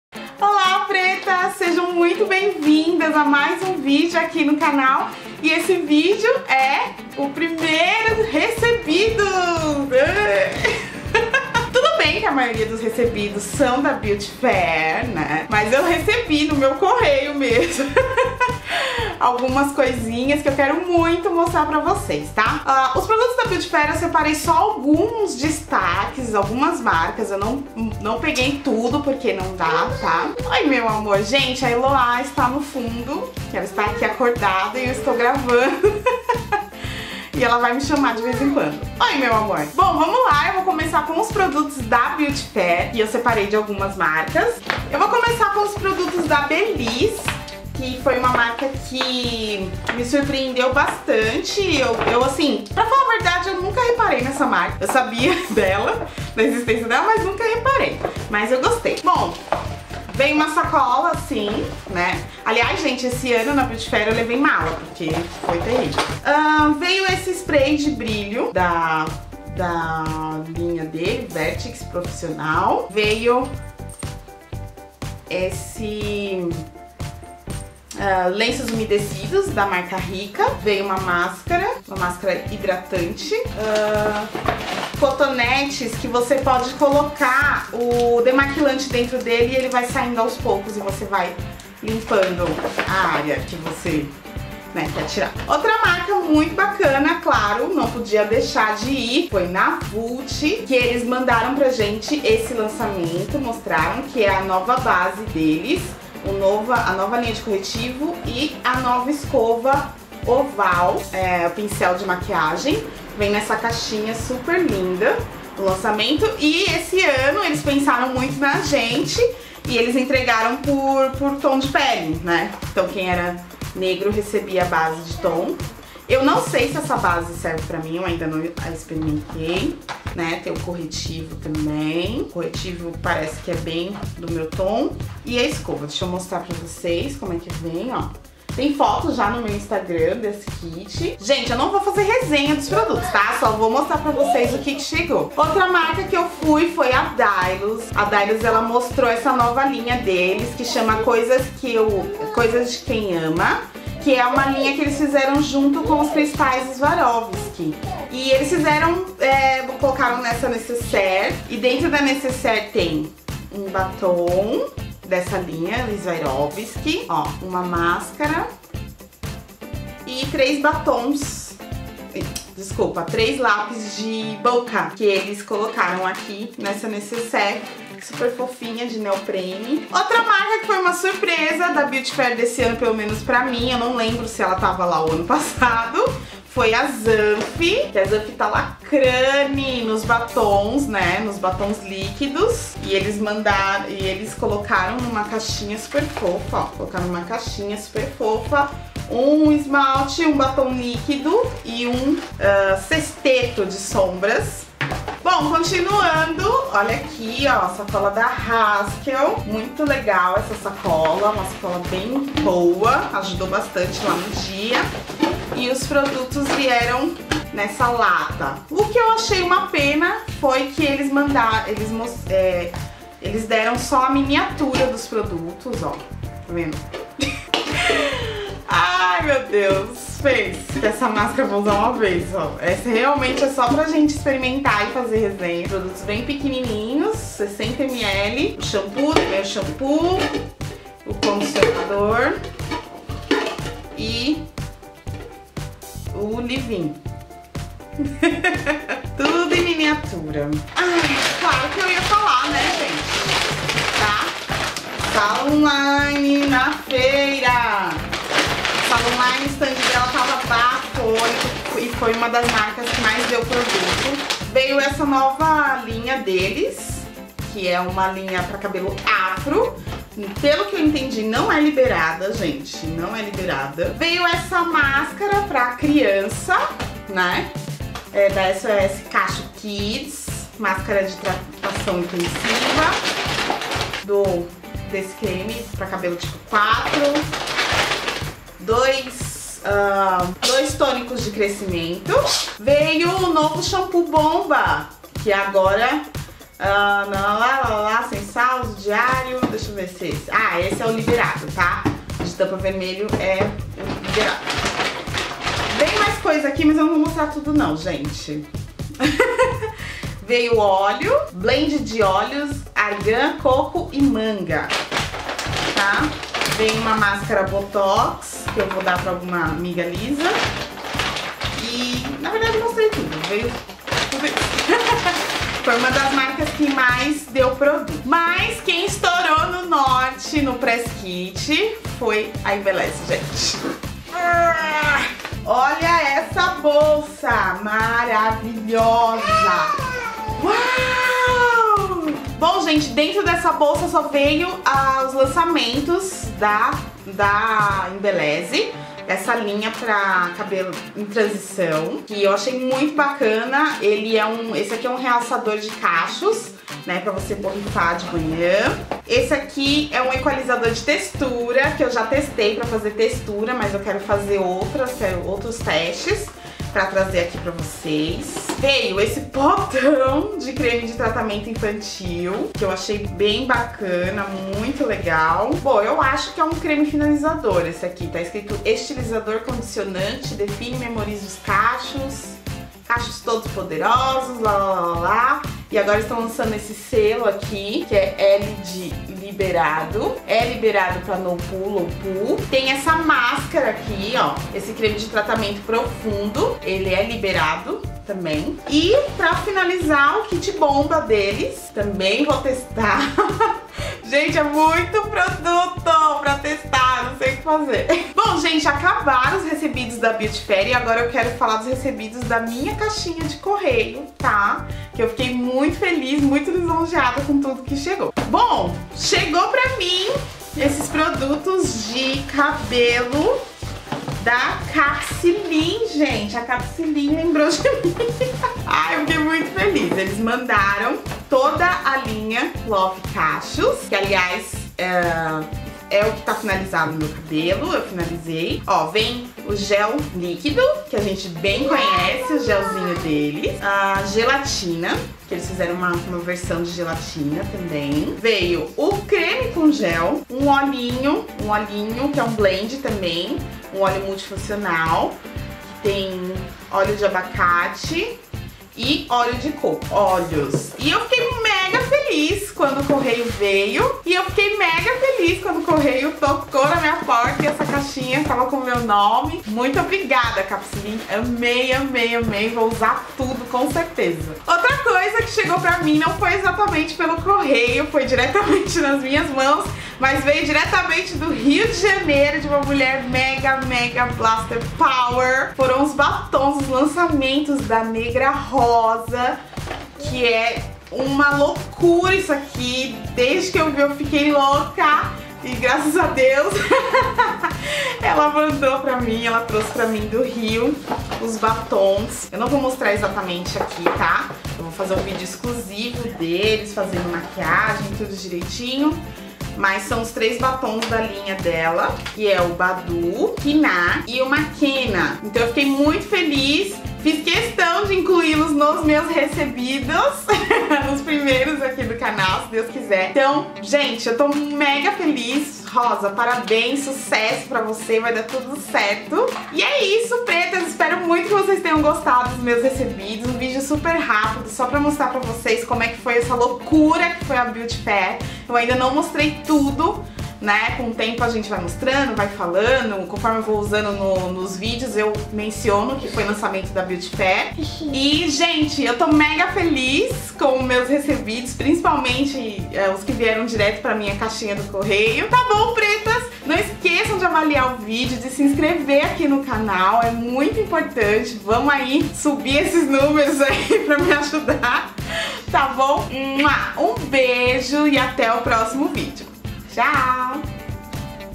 Primeiro Recebidos: Beauty Fair, Capicilin, Negra Rosa e mais! | Patrícia Avelino
Pretas!

Estou mega feliz em mostrar pra vocês meus primeiros recebidos!

Aperta o play!